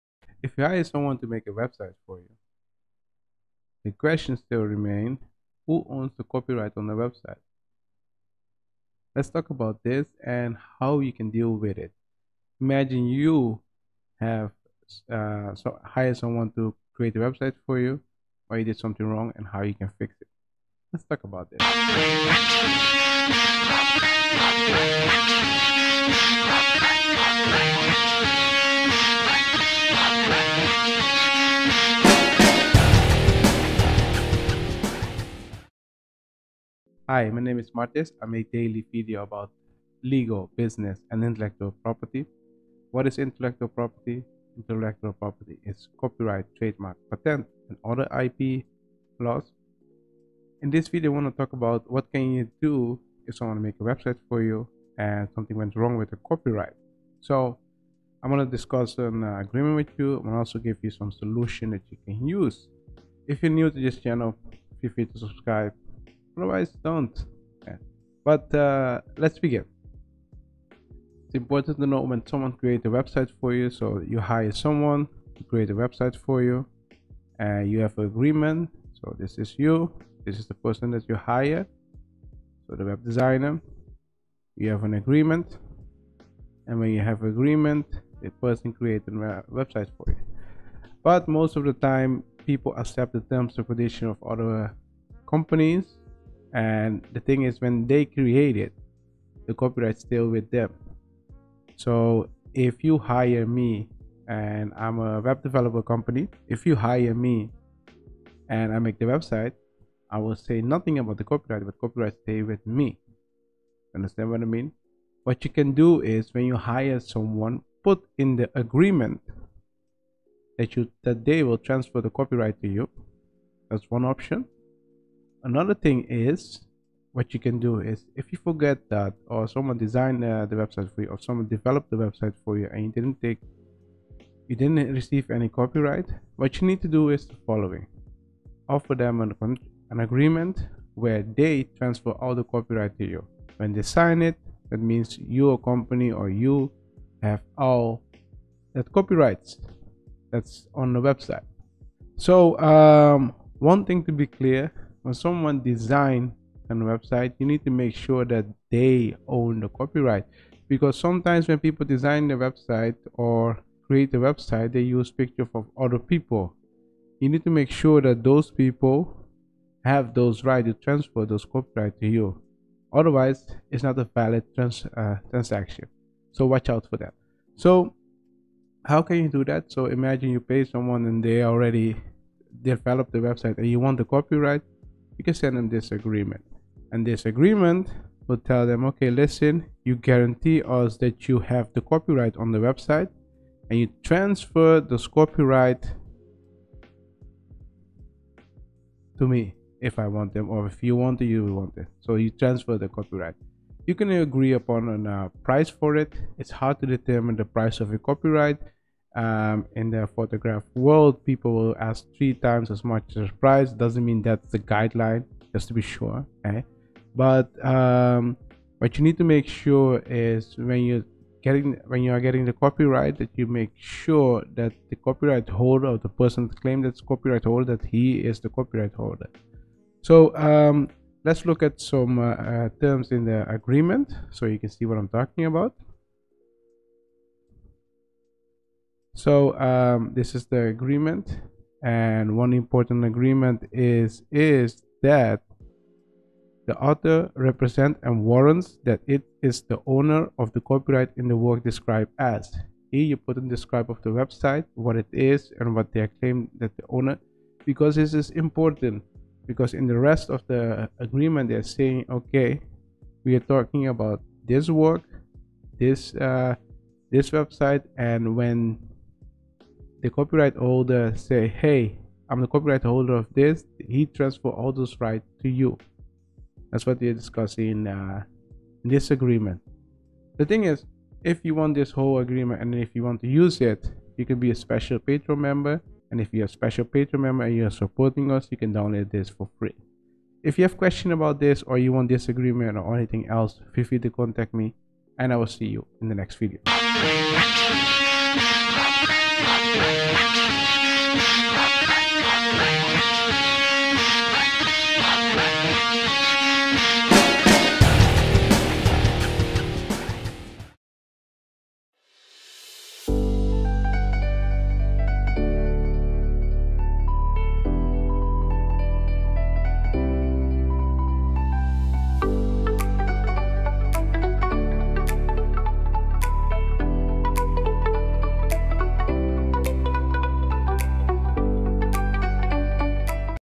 ASSIGNMENT OF COPYRIGHTS (FOR WEB SITE CREATOR) | Martisz Legal
In today's video, we're discussing the assignment of copyright for website creators.

Official Websites